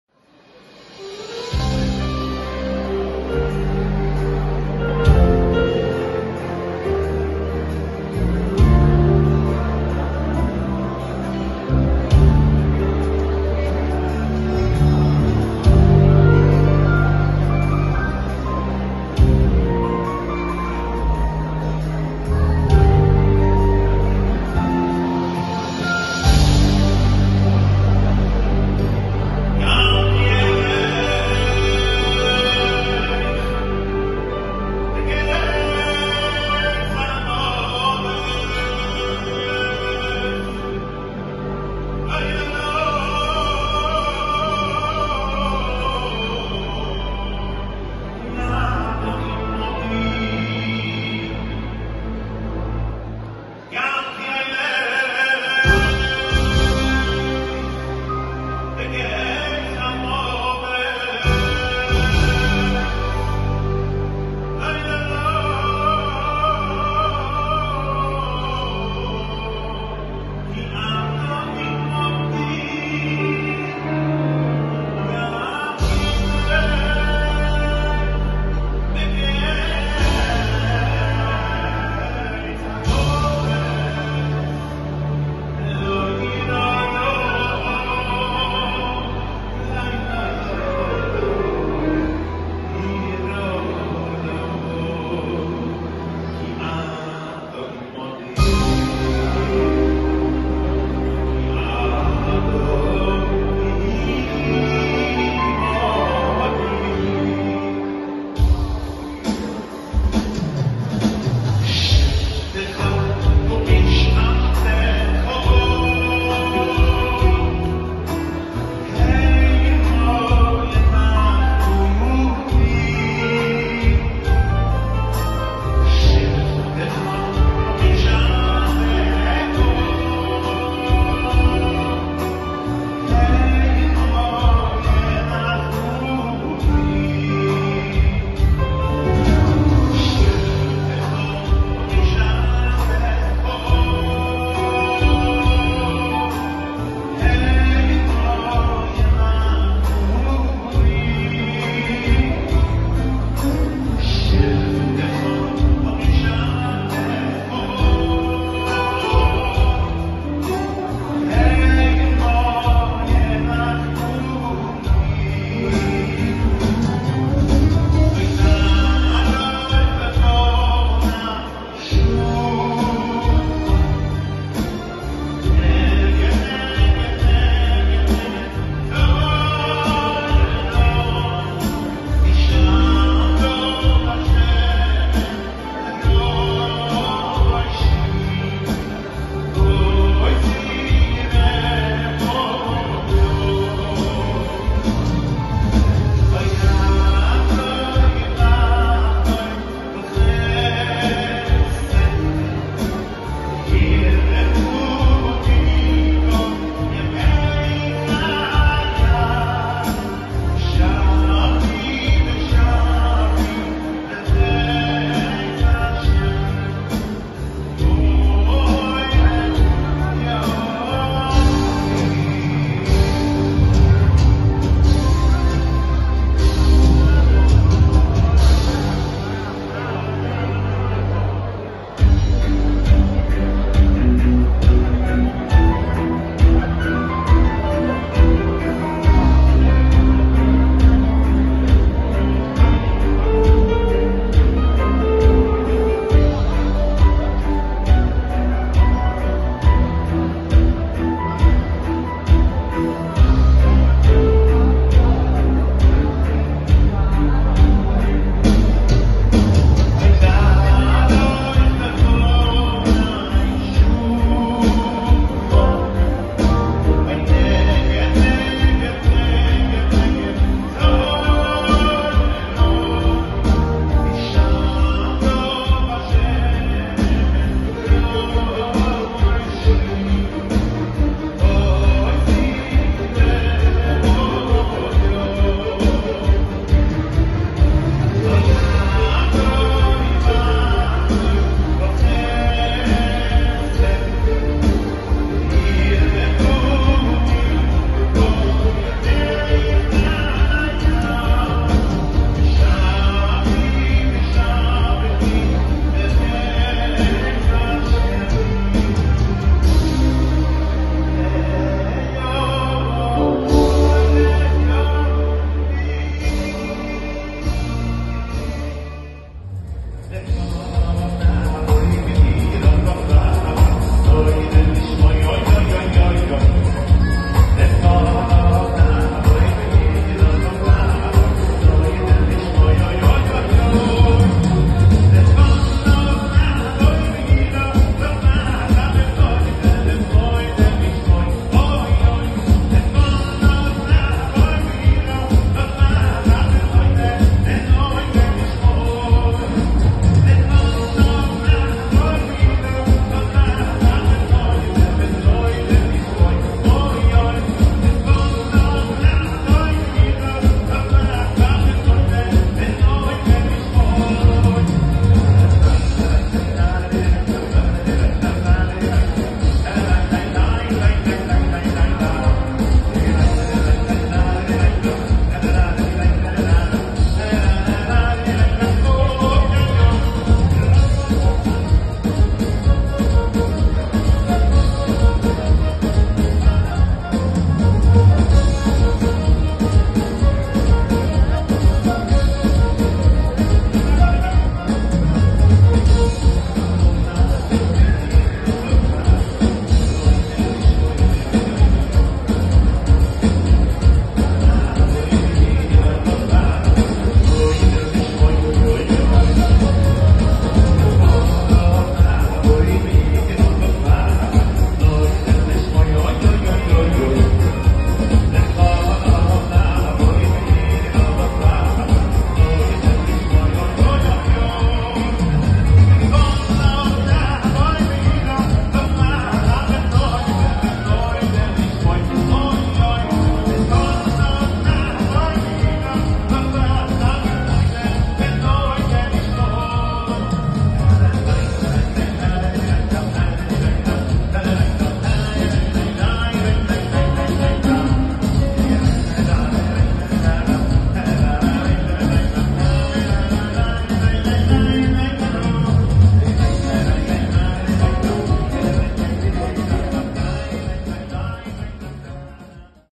Efrayim Mendelson the power- greatest current jewish singer today - אפרים מנדלסון עם להיטים גדולים!
Singing hits like only he can!  Thank you Reb Efryaim